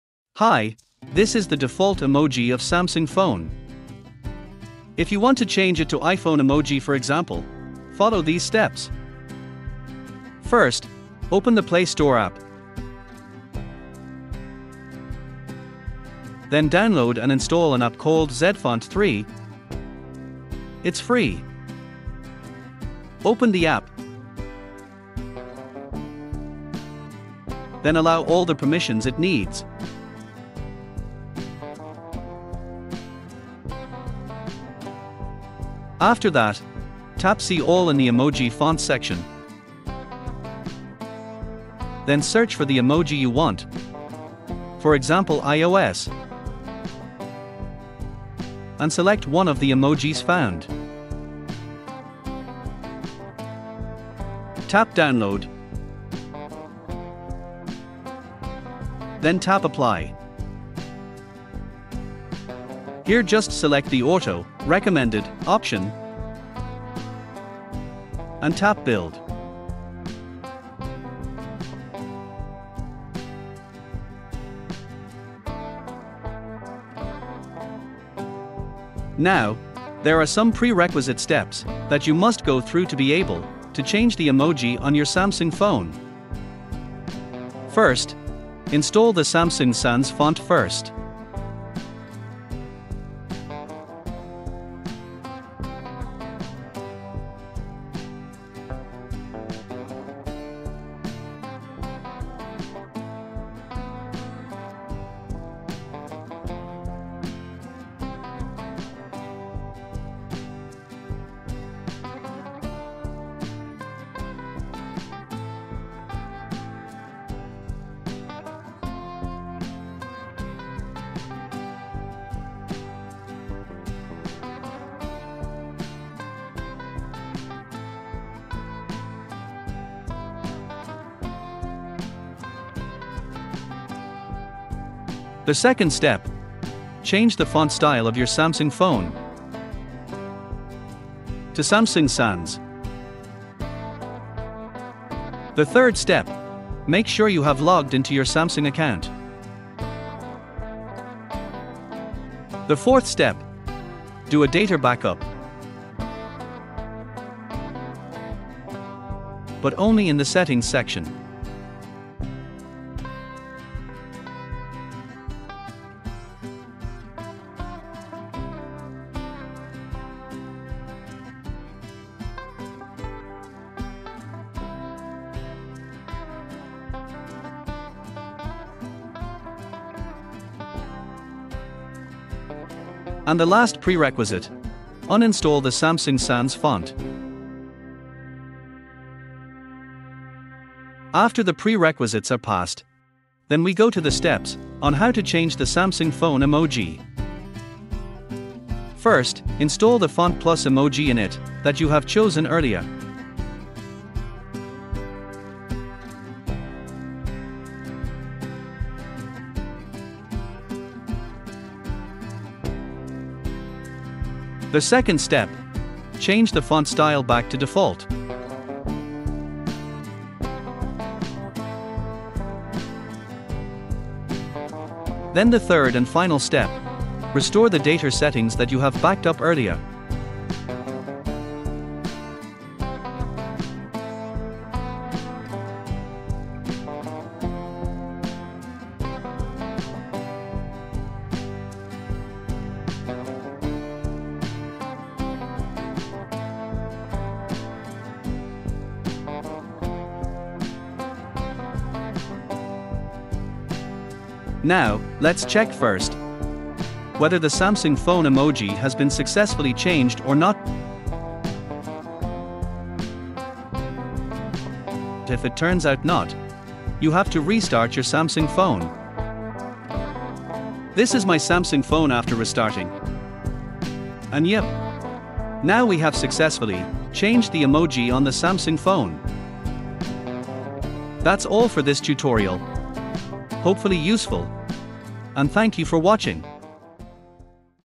How to Change Samsung Emoji to iPhone Emoji Using zFont 3 - Easy Steps!
Samsung emoji to iPhone emoji. Want to change the look of your Samsung emojis to match the stylish iPhone emojis? In this tutorial, I'll show you how to do it using the zFont 3 app.

It's a straightforward process, but there are a few steps you'll need to follow before you can successfully switch your Samsung emoji style. Don't worry, though—I'll guide you through each step, making it easy to transform your emojis. Watch the video to learn how!

Video Highlights:
1. Introduction to zFont 3
2. Step-by-step guide to changing Samsung emojis
3. Easy-to-follow instructions
4. Tips and tricks for a smooth transition